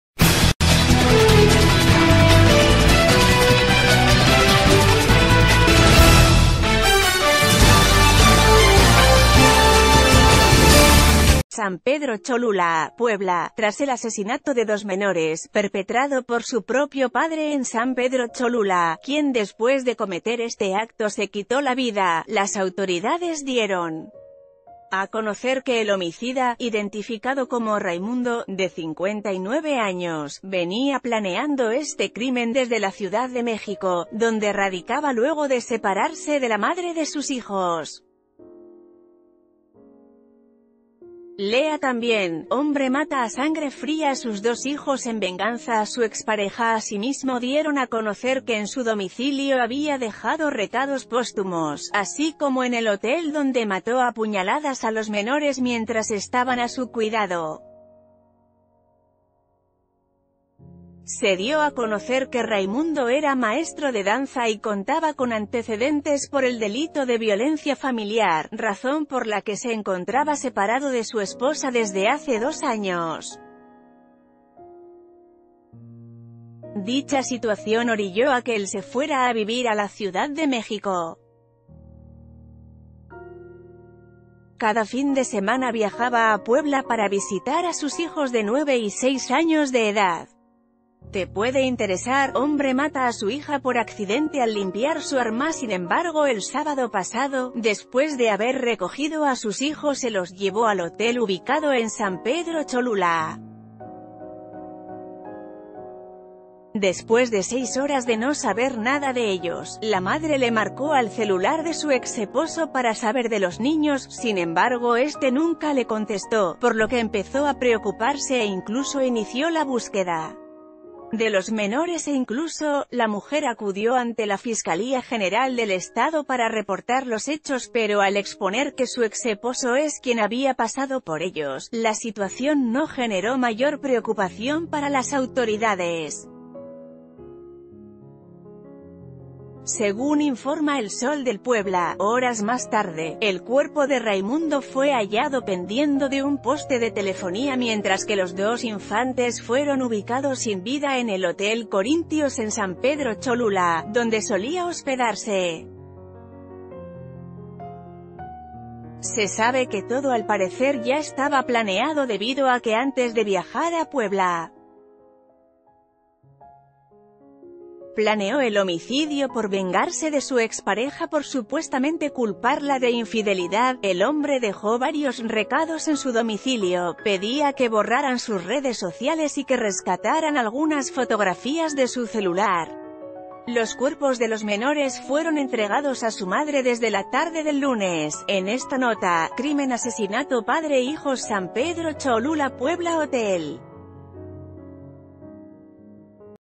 Revelan razón por la que papá mató a sus hijos de 9 y 6 años
San Pedro Cholula, Puebla.- Tras el asesinato de dos menores, perpetrado por su propio padre en San Pedro Cholula, quien después de cometer este acto se quitó la vida, las autoridades dieron a conocer que el homicida, identificado como Raymundo, de 59 años, venía planeando este crimen desde la Ciuda...